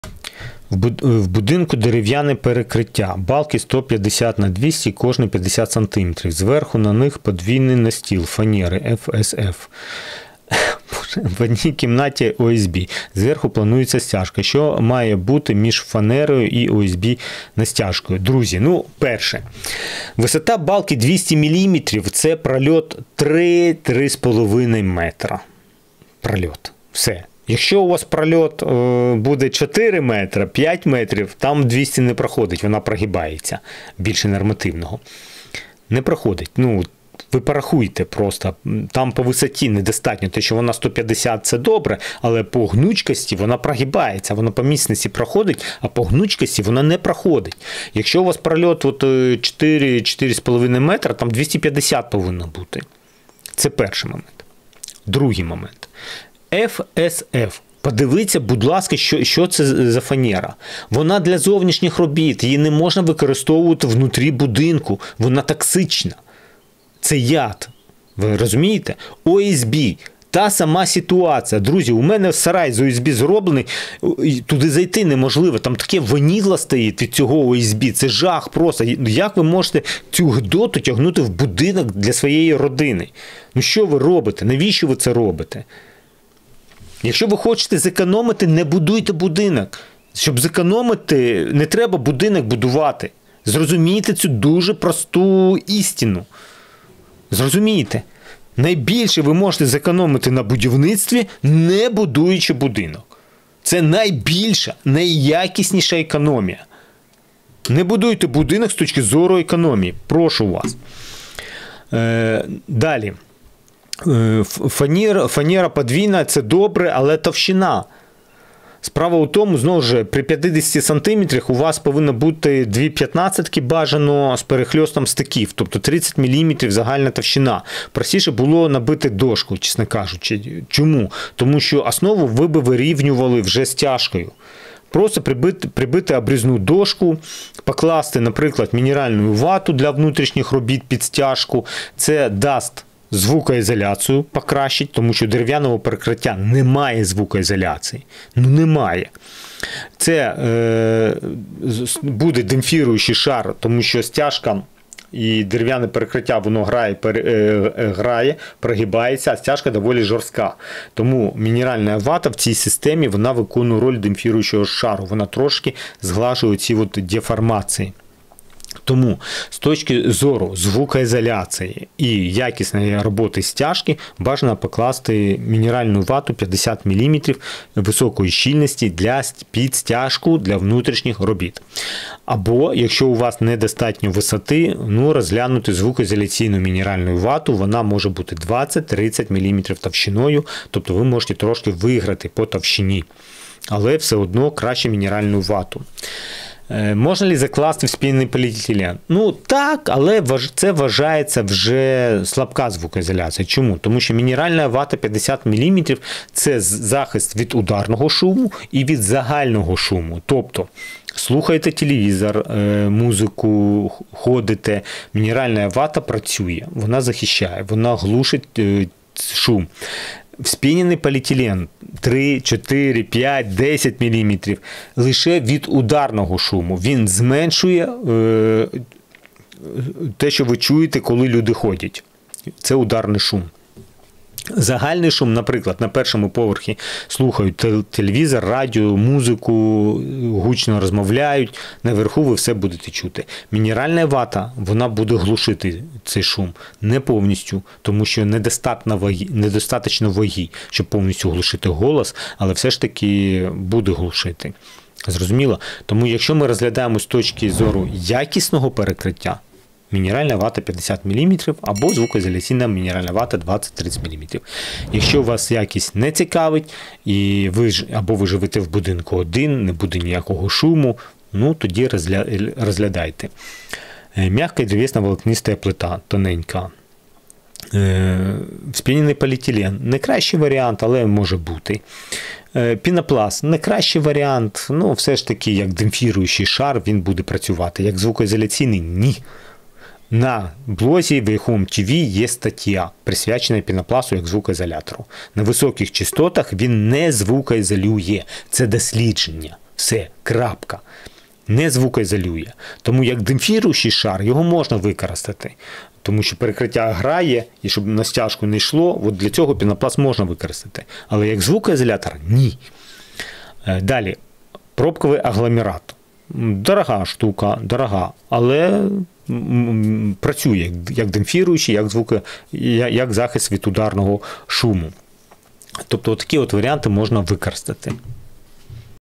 Перекриття з отрутою для всієї родини! Беріть дешево!
ФК екологічно безпечна, не вологостійка, досить крихка, швидко розшаровується та ламається під впливом вологи. Застосовується для внутрішнього оздоблення, у виробництві меблів. ФСФ негативно впливає на здоров'я, стійка до вологи, має підвищену міцність до тиску зламу.